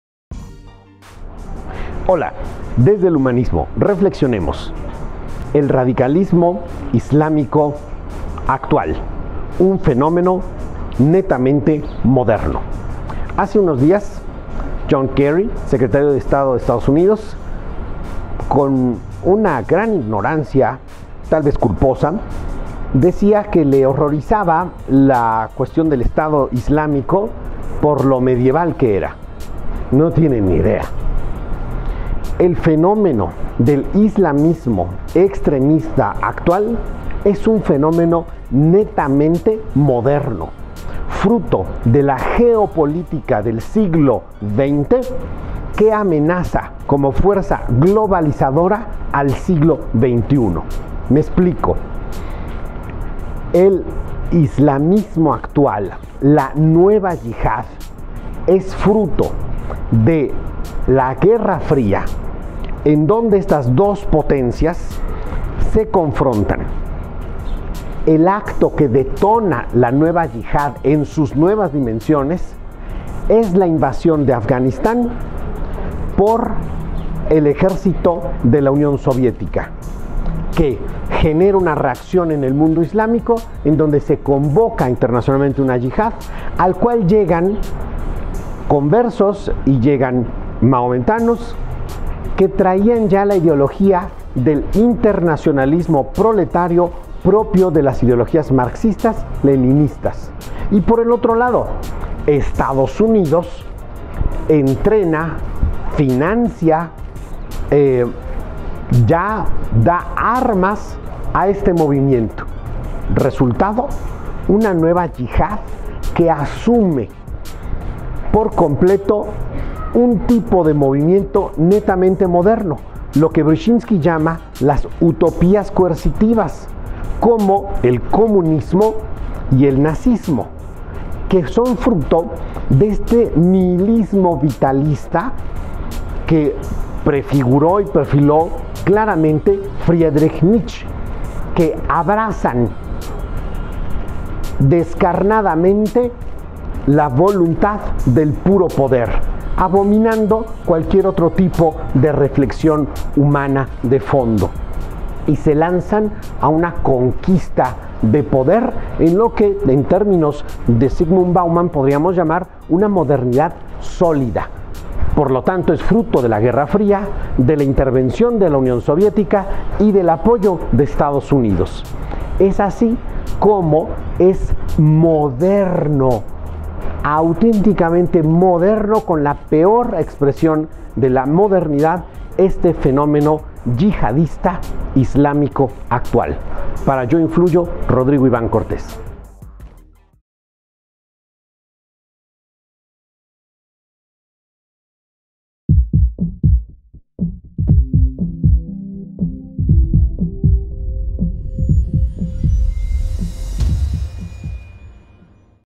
Islamismo extremista, fenómeno moderno que amenaza al siglo XXI
Se equivoca John Kerry: el Estado Islámico no es un fenómeno medieval, sino moderno, fruto de la geopolítica del siglo XX.